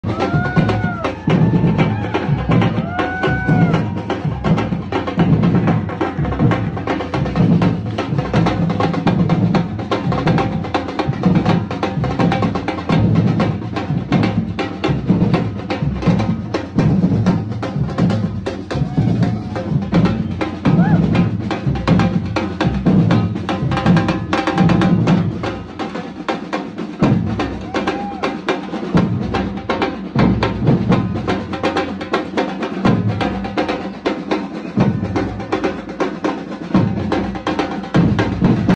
Stella Nyanzi at FCKAFD protest in Bavaria
Although I am not a German, I joined the countrywide mass protests in Germany because democracy must continuously be protected and defended.